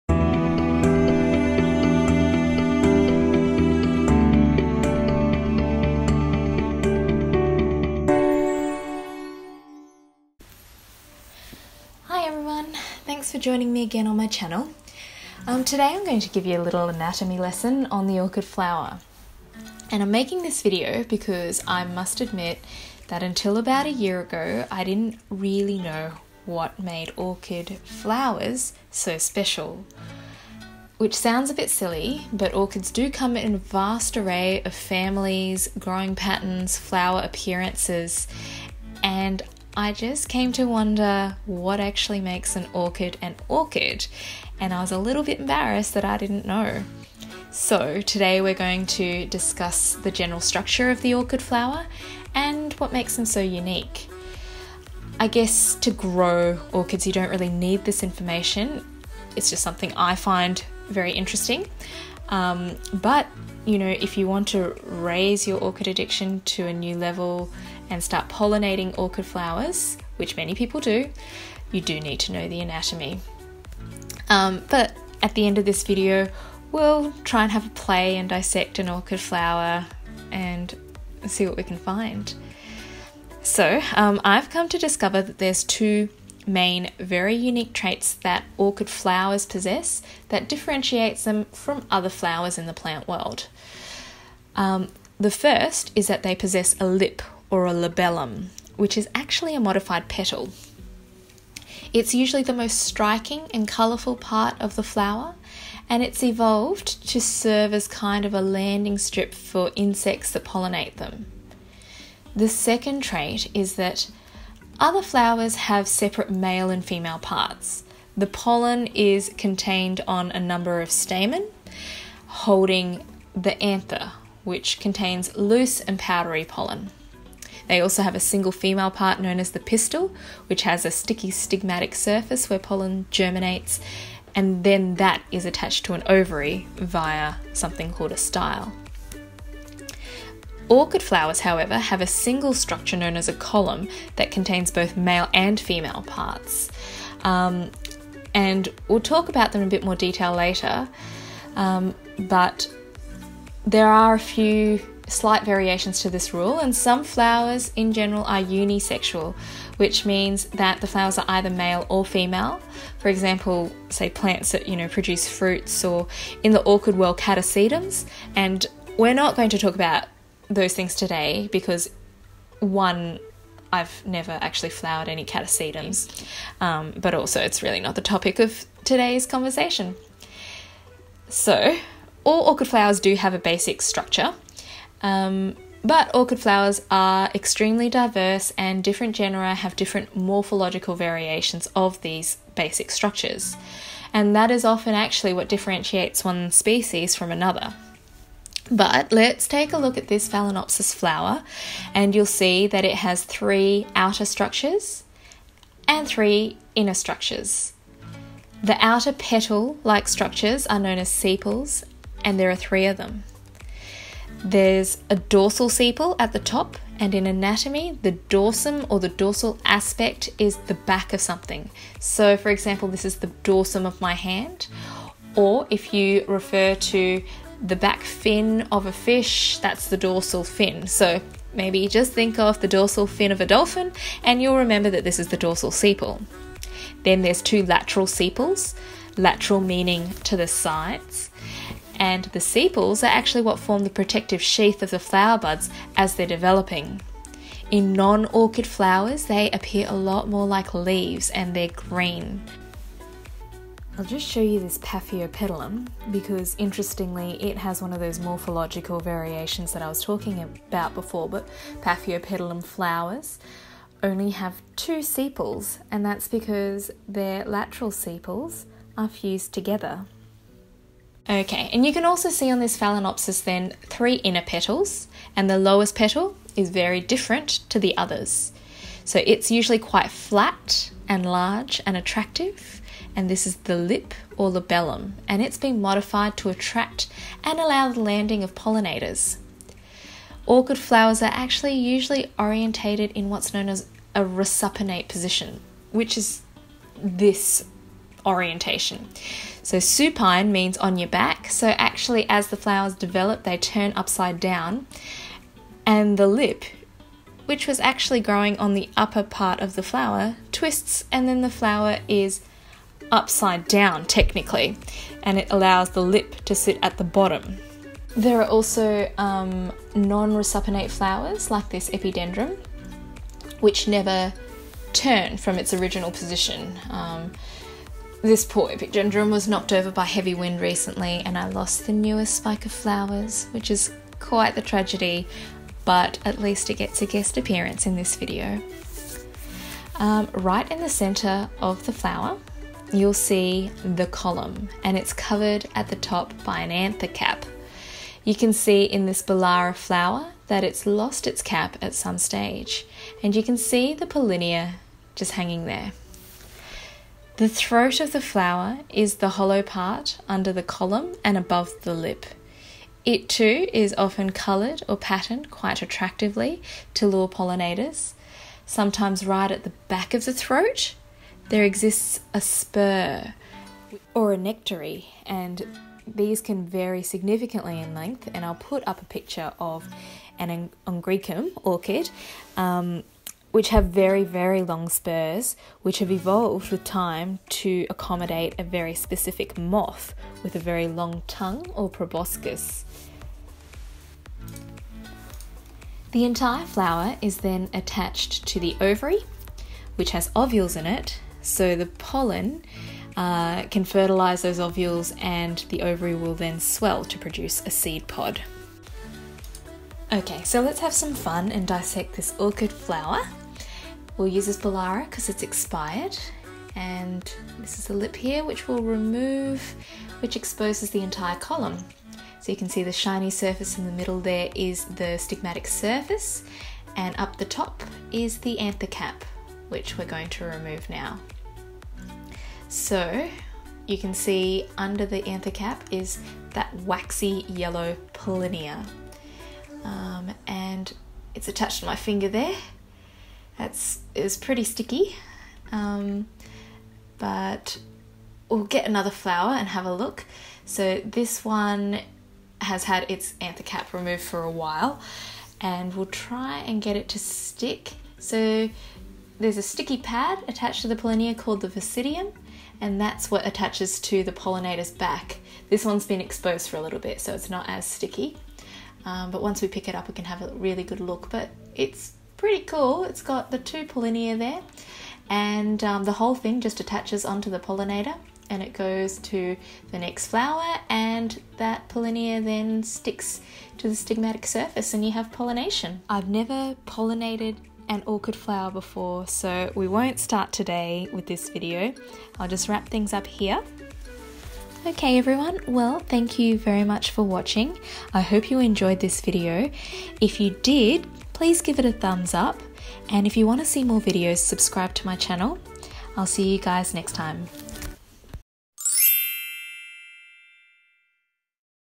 Orchid Flower Anatomy
Orchid flowers vary so vastly between the different genera - but what do they have in common? What makes them ... orchids?!  I wondered this last year and felt a little silly I had all these orchids and didn't know why they were classified as orchids in the first place.

Anyway, this is very much a beginner's video.  Looking at the main parts of the orchid flower, talking about what makes them so unique, and a few interesting facts thrown in along the way.

We'll also pull apart a flower at the end and talk about what we find.  Hope you all enjoy!!  Sorry if it's too basic for most of you, but I still had fun discovering and making this video ;).